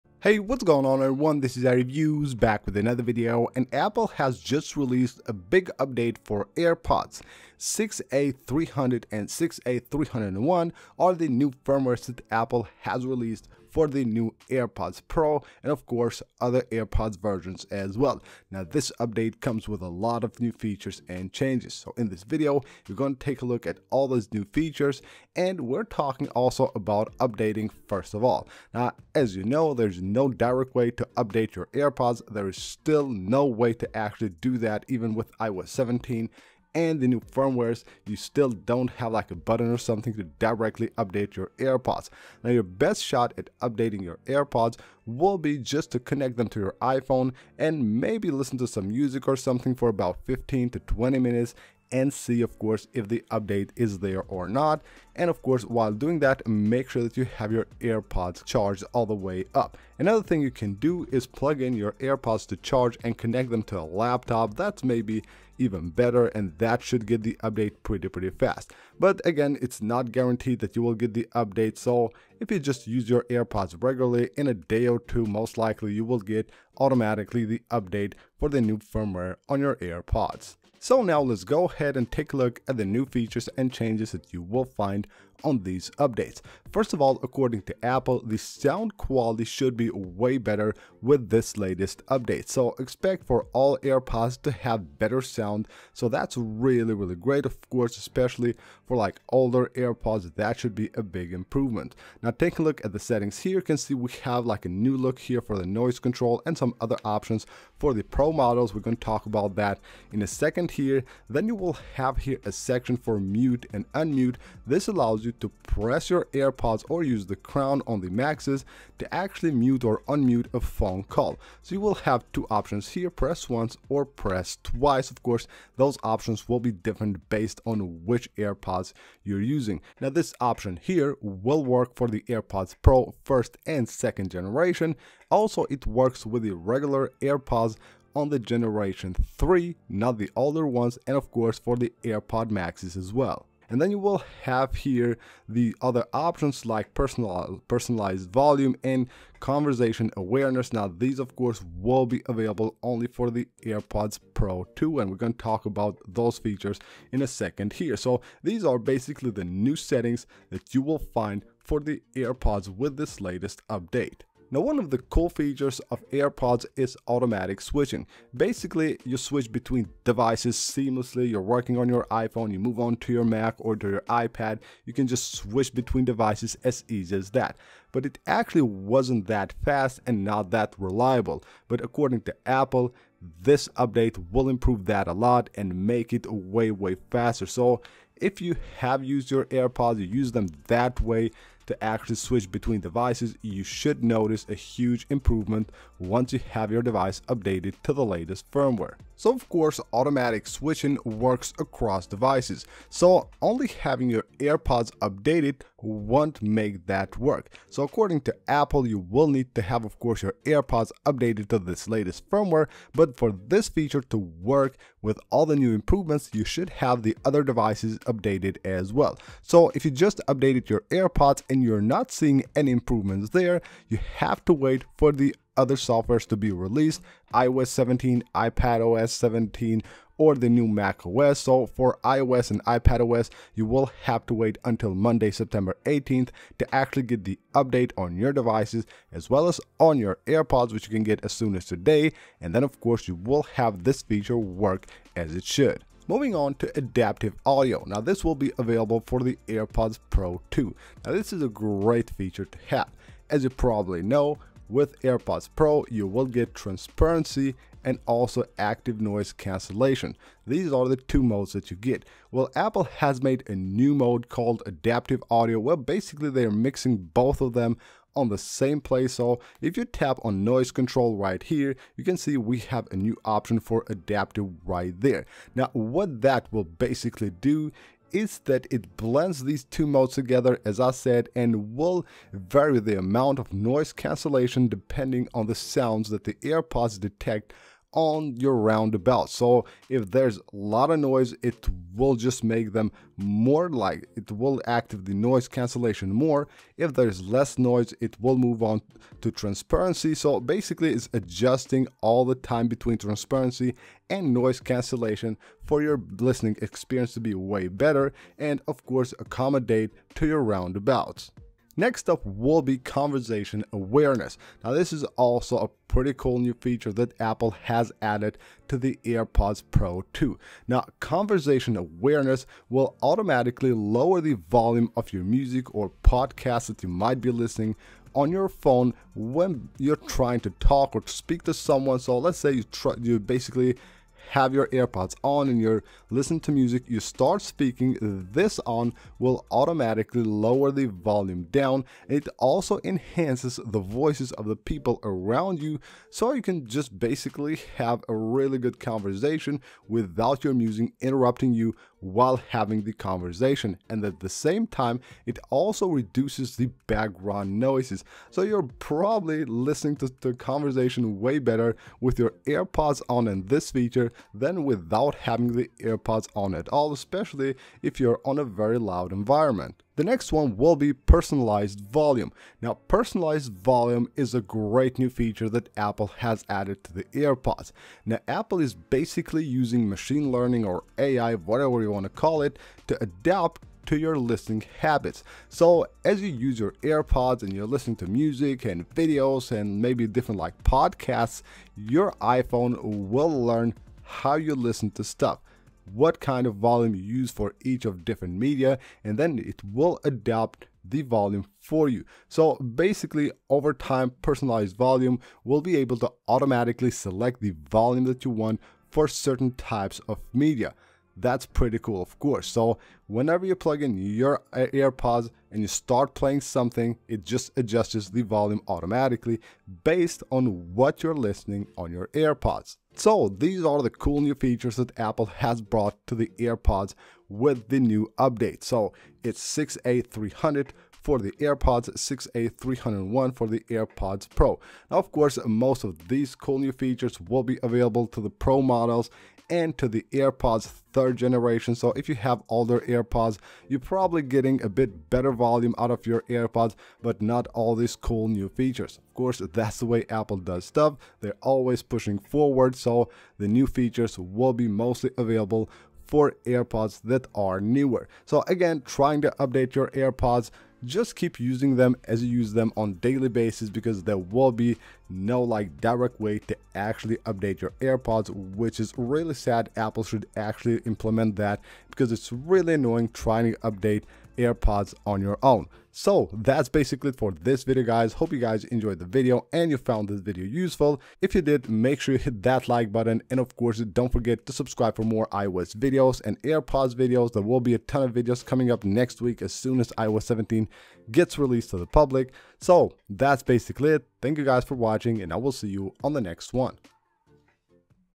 AirPods Update 6A300 / 6A301 Released - What's NEW !?
AirPods & AirPods Pro new firmware 6A300 / 6A301 have been released with new features and changes making this a great new update for AirPods users.

In this video you can learn more about the 6A300 / 6A301 update, all the new features and how to try to install this update faster on your AirPods.

How to install 0:00
New Features 1:20